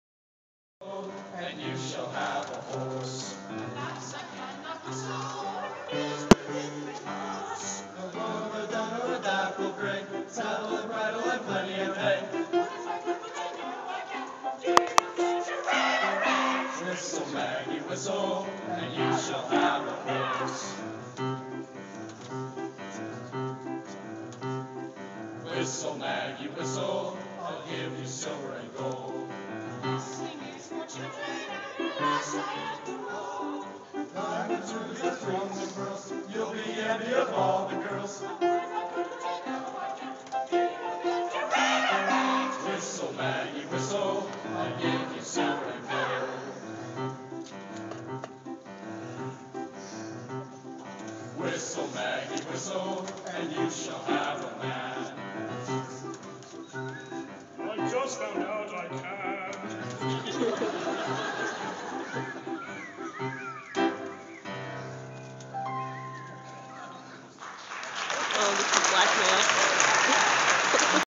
Marsh valley mantastics
This is the men's choir from Marsh valley. My friend/ friends boyfriend is the one on the left wearing a skirt. :-) enjoy